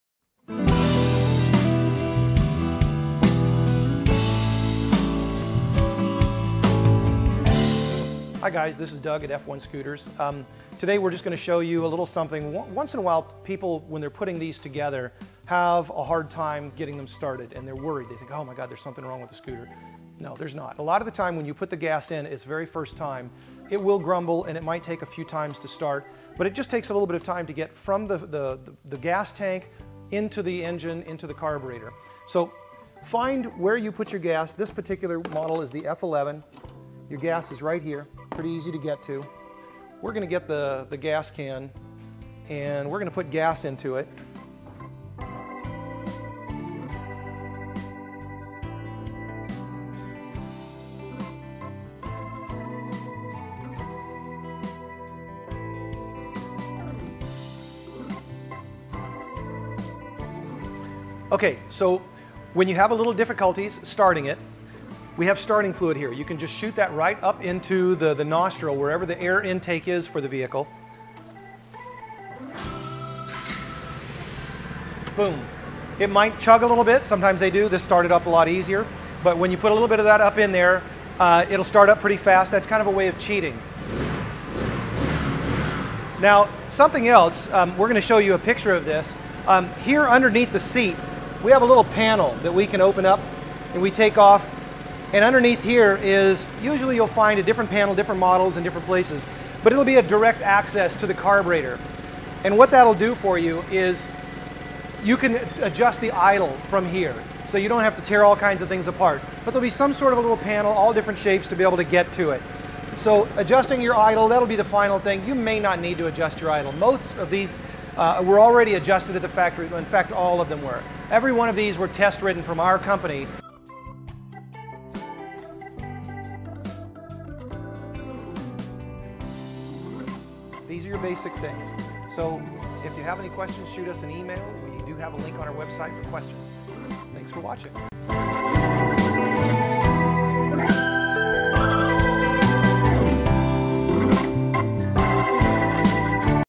F1 Scooters Presents: GY6 50cc-150cc First Time Starting Your Scooter!
This video shows you how to start your brand new F1Scooters scooter. This also pertains to 50cc-150cc Scooters of all models and makes.